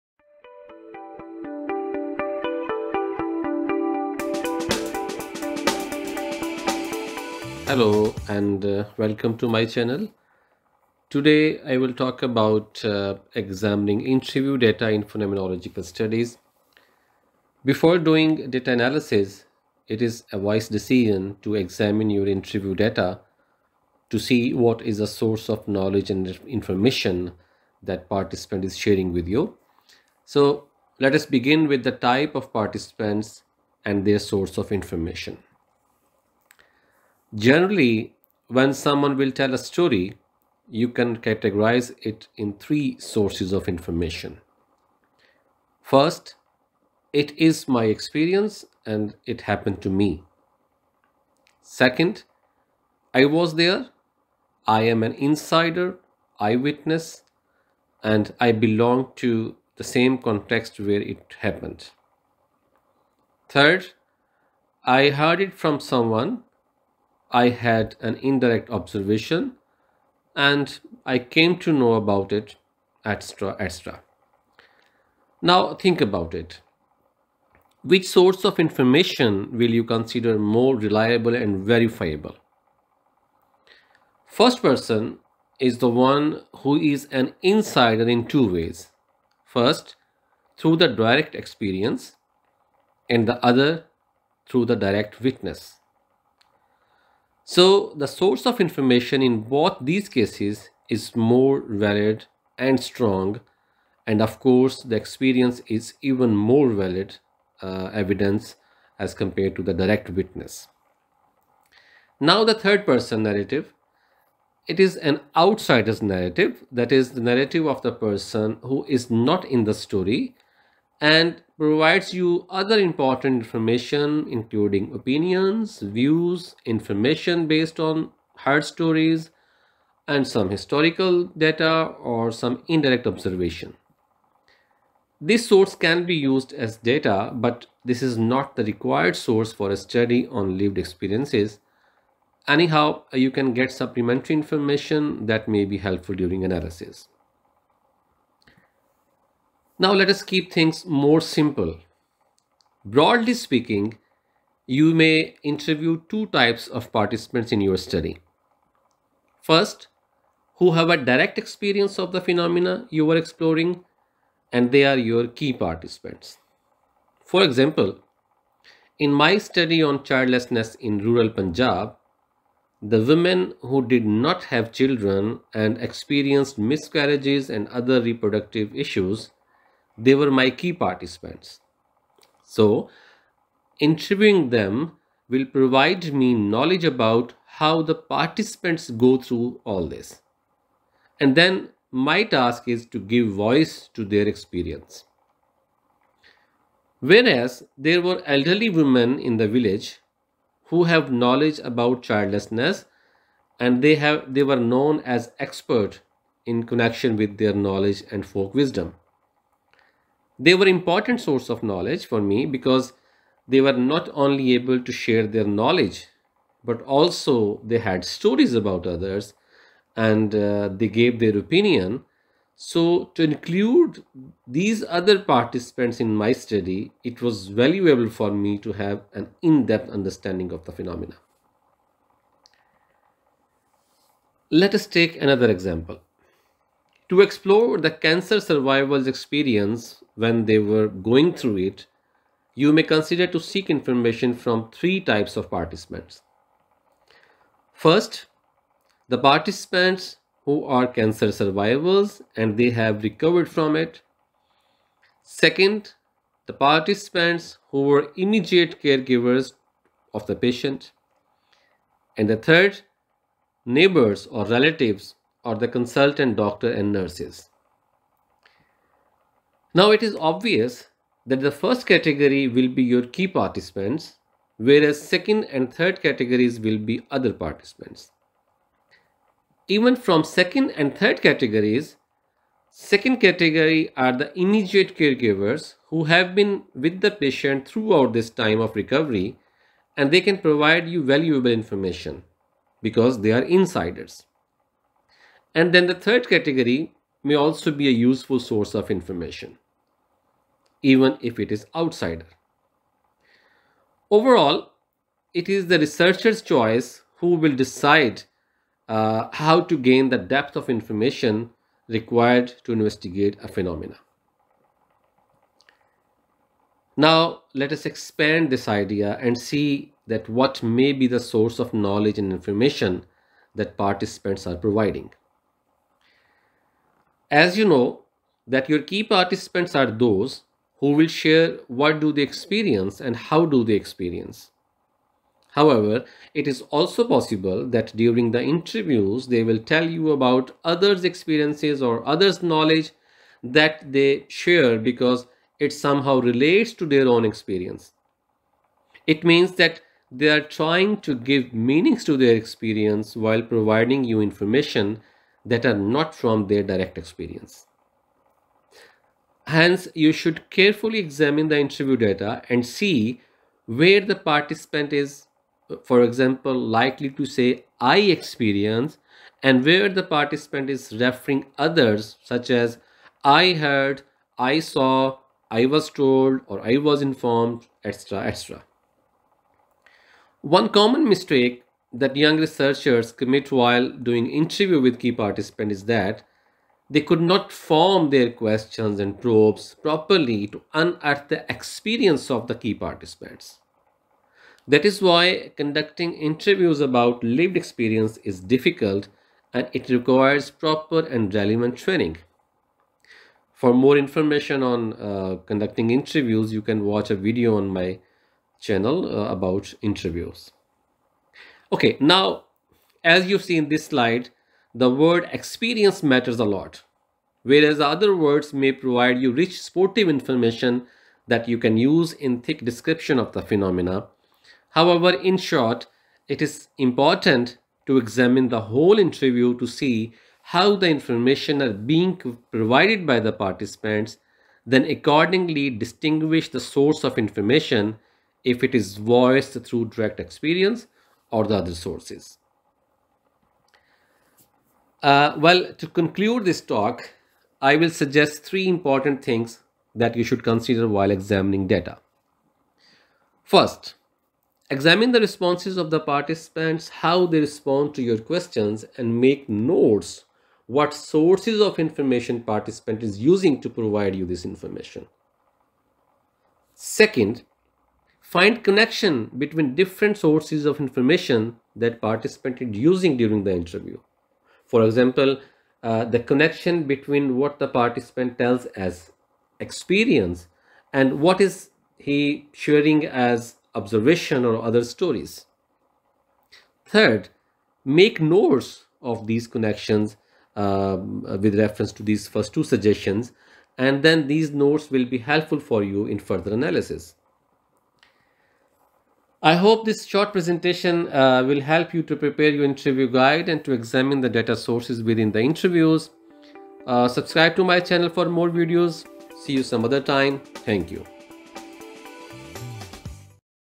Examining interview data in phenomenological studies
examining interview data in phenomenological studies - types of participants and source of information.